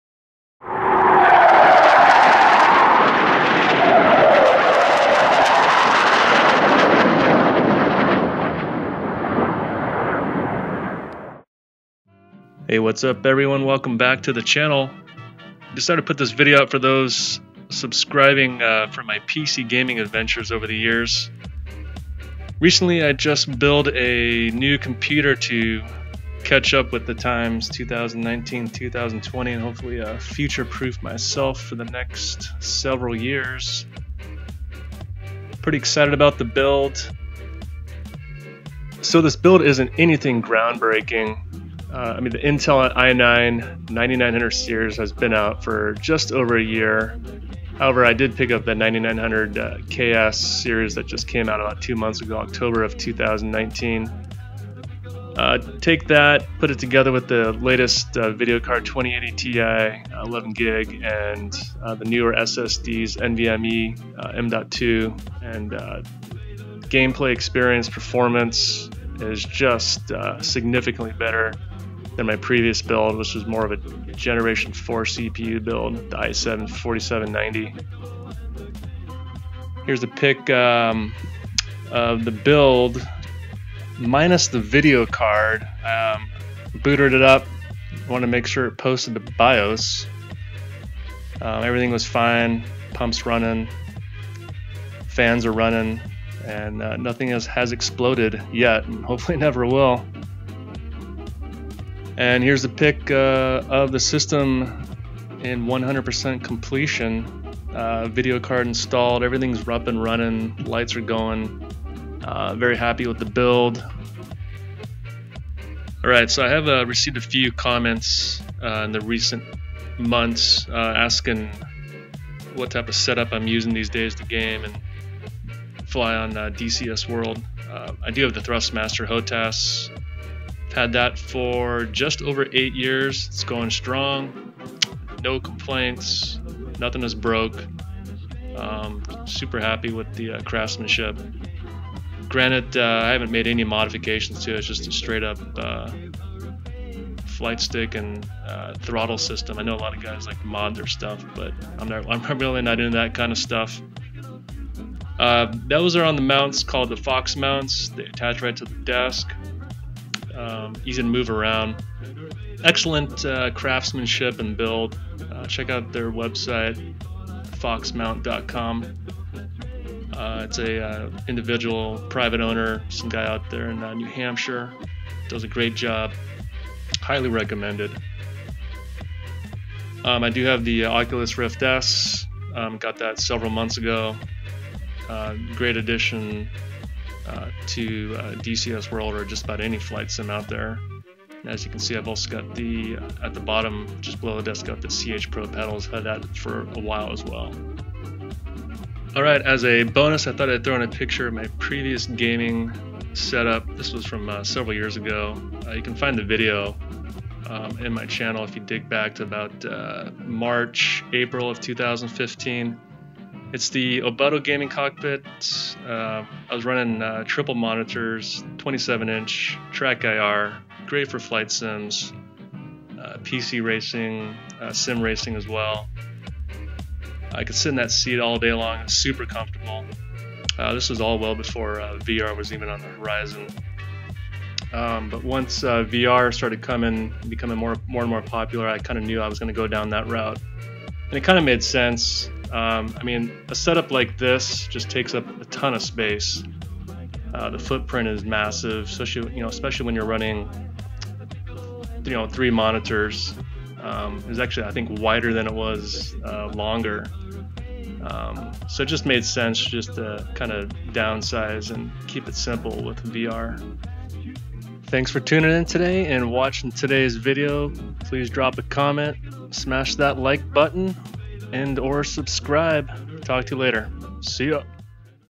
New computer i9 9900KS, 2080 Ti, NVMe M.2 SSD, VR
I recently finished building a new computer.  Is it already obsolete?! :P

0:38 - Quick rundown of the new specs
1:18 - A few pics of the new build
1:56 - Pics and discussion of the hardware I use for PC Gaming and DCS World
3:18 - Remembering my old rig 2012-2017 (Triple monitor setup, Obutto rig)

Full specs plus gaming peripherals-

Corsair 750D Obsidian Airflow Edition
EVGA 2080Ti XC Ultra 11GB
i9 9900KS Special Edition
Corsair H150i Cooler
SSDs: 2 x NVMe M.2 970 EVO PLUS 1TB, Crucial 512GB, Samsung 850 EVO 500GB
Corsair 1000W power supply
Speakers: Bose Companion 3
Razer Death Adder 3.5G
Track IR 4 Pro
Thurstmaster Warthog, MFDs, and CH Pro Pedals
TM T500 RS w/ Leather 28 GT and custom F1 wheels
Viotek 34” 1440p monitor